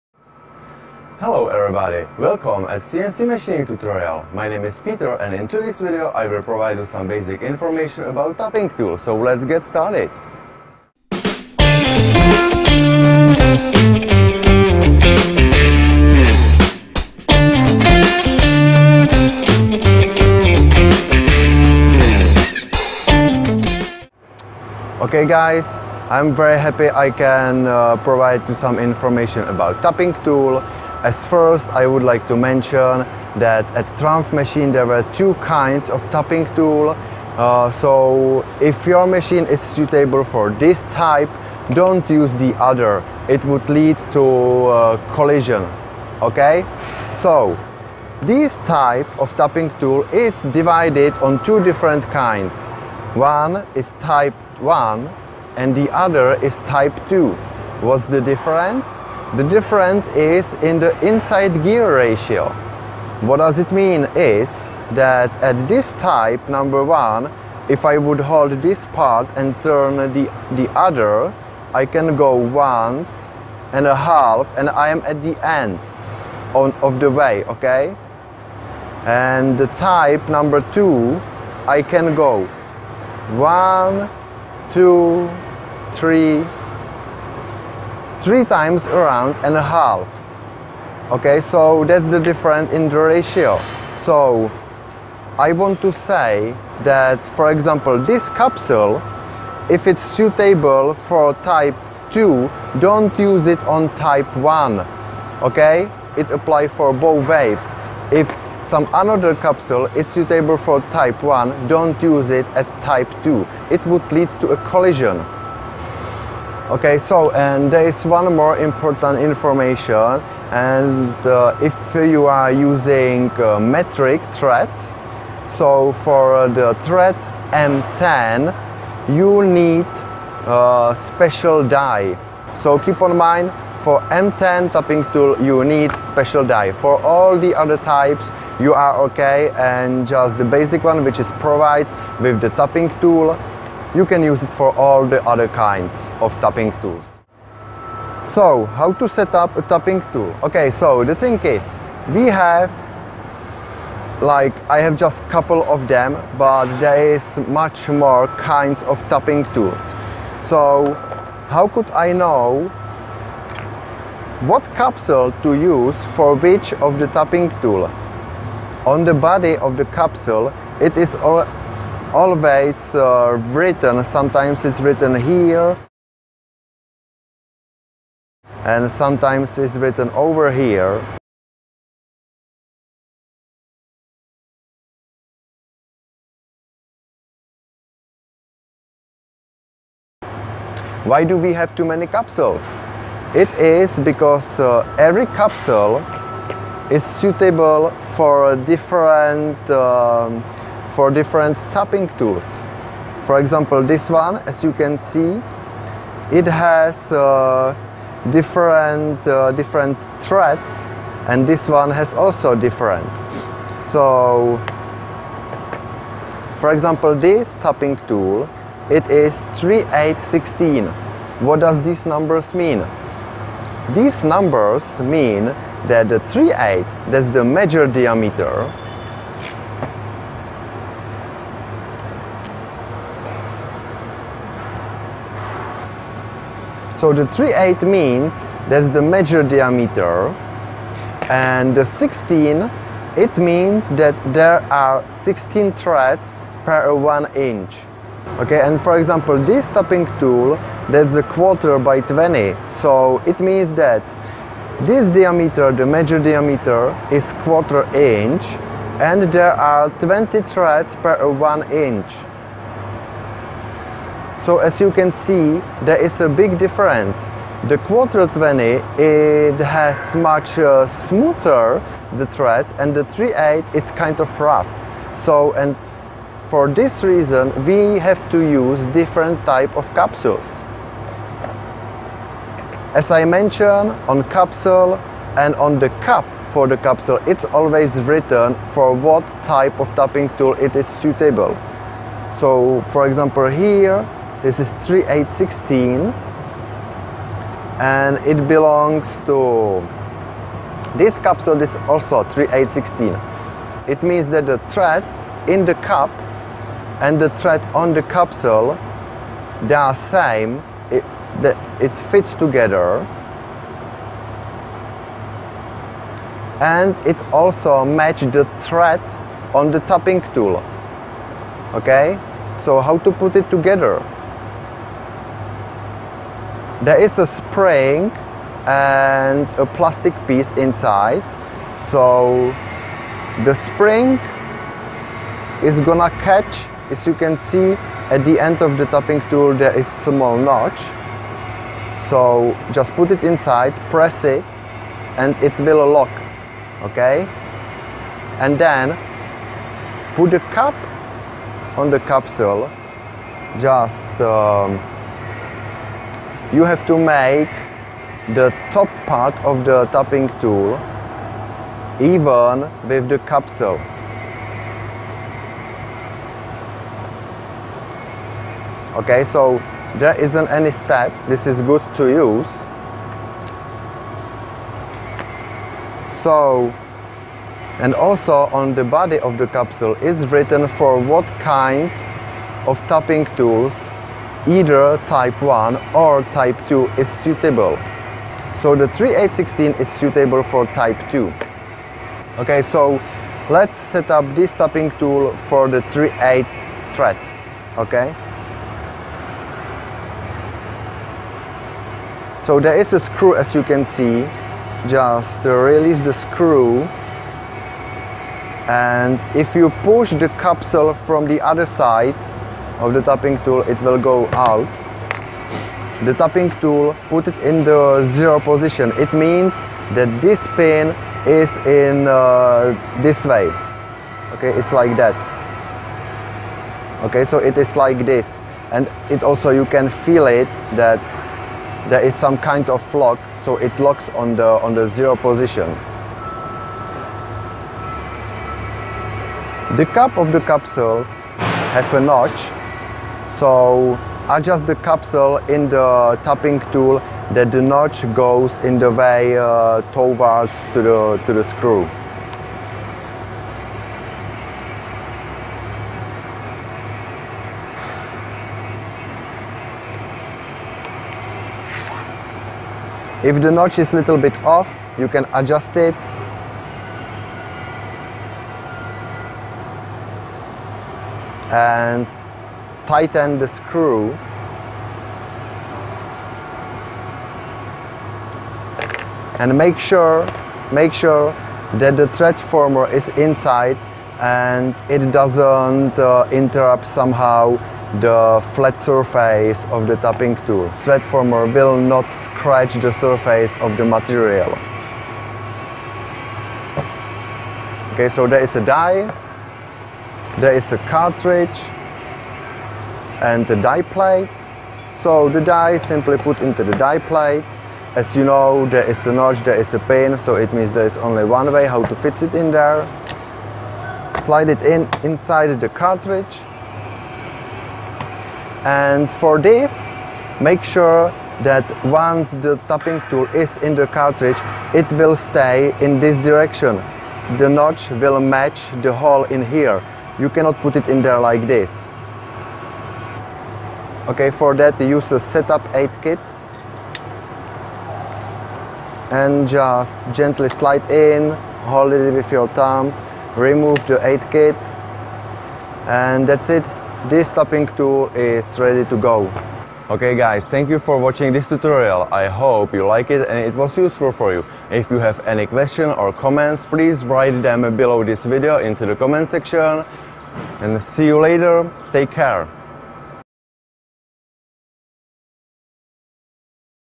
Tapping Tools for Trumpf Trumatic punch press machines
How to setup tapping tools for Trumpf Trumatic punch press machine.
This video is cnc machining tutorial about punching tools which are used at Trumpf cnc punch press machines.
We will focus on how to assemble a tapping tool.

This tutorial has been made with tools for machine Trumpf 7000.
For older/newer machines, the tools can vary.

If you have any comments or questions, please write them bellow.
Petr